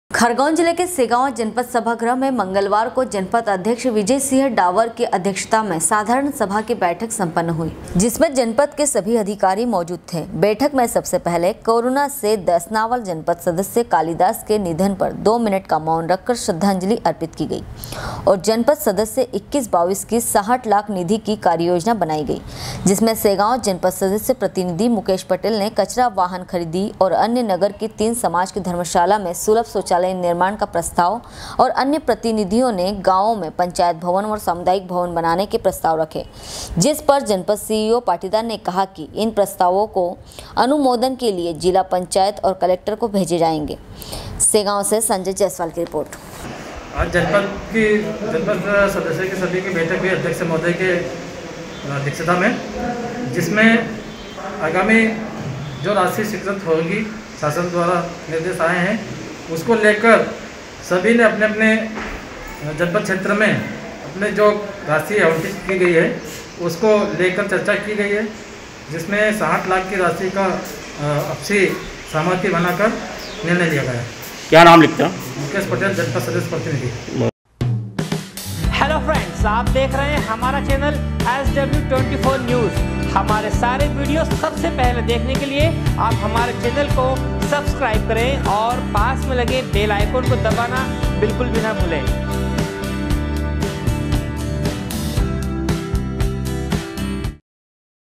segaon- खरगोन जिले के सेंगावा जनपद सभागृह में मंगलवार को जनपद अध्यक्ष विजय सिंह डावर
खरगोन जिले के सेंगावा जनपद सभागृह में मंगलवार को जनपद अध्यक्ष विजय सिंह डावर की अध्यक्षता में साधारण सभा की बैठक सम्पन्न हुई। सेगांव से संजय जायसवाल  की रिपोर्ट